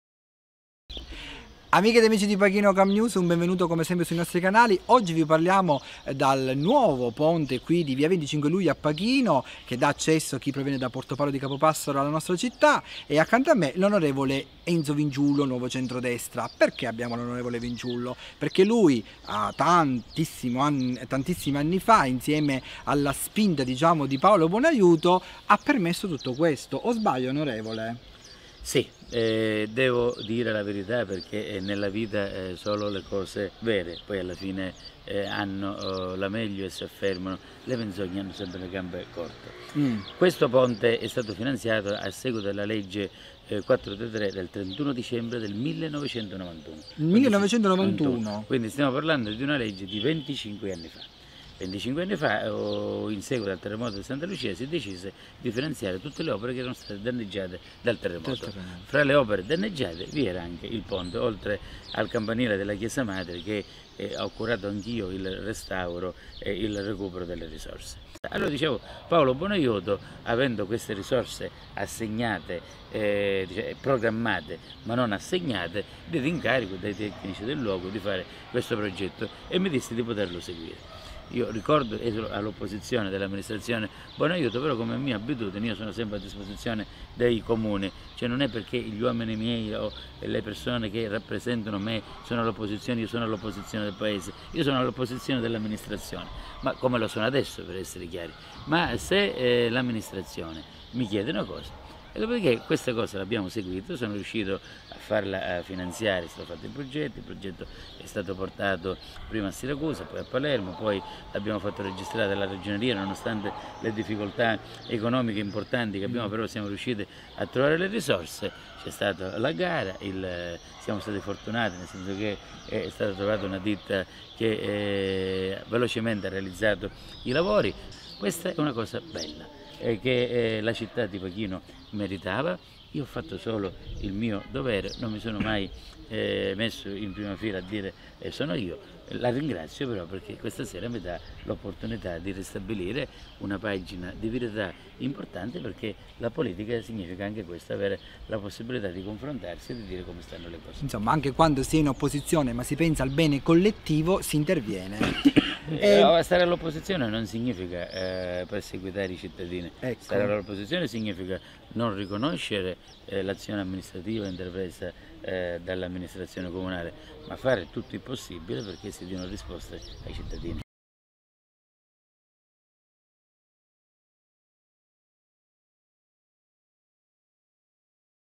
Il ponte, Vinciullo e le menzogne dalle gambe corte.
A Pachino il ponte di via XXV Luglio si rifà il look. L'Onorevole Enzo Vinciullo racconta come si è arrivati a questo successo iniziato nel 1991. Guarda questo video in HD.